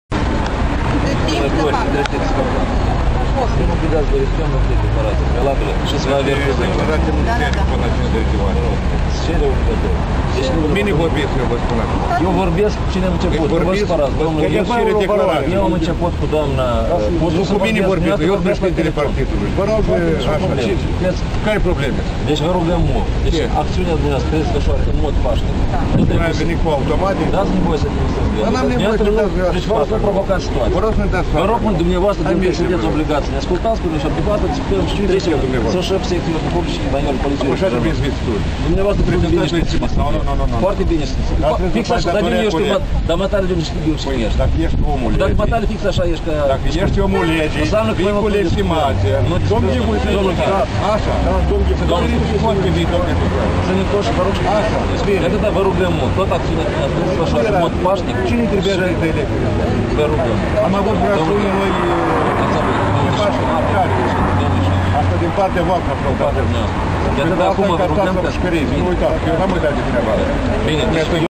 Politia avertizeaza liberalii sa manifesteze in mod pasnic
Politia avertizeaza liberalii sa manifesteze in mod pasnic - 28 iunie 2008